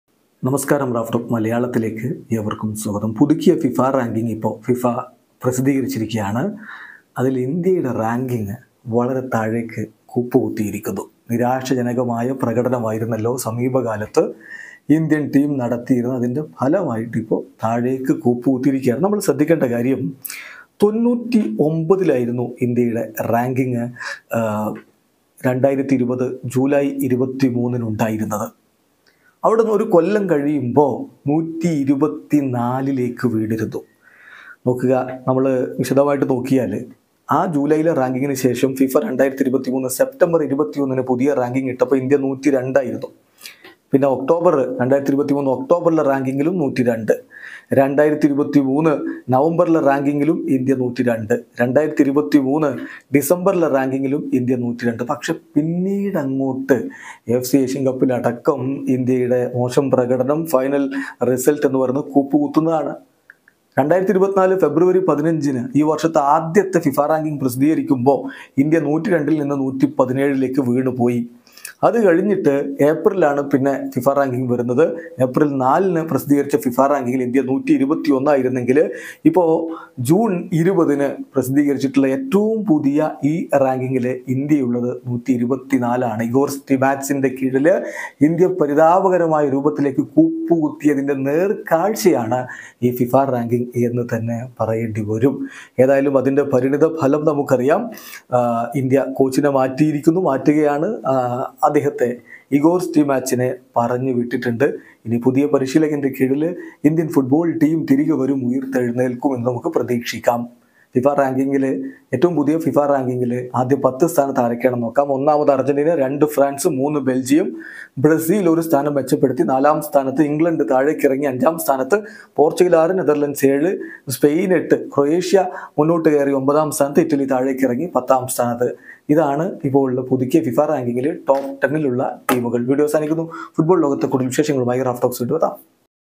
പുതുക്കിയ ഫിഫ റാങ്കിംഗ്:ഒന്നാമത് അർജൻ്റീന,കൂപ്പ് കുത്തി ഇന്ത്യ | FIFA Ranking
FIFA Ranking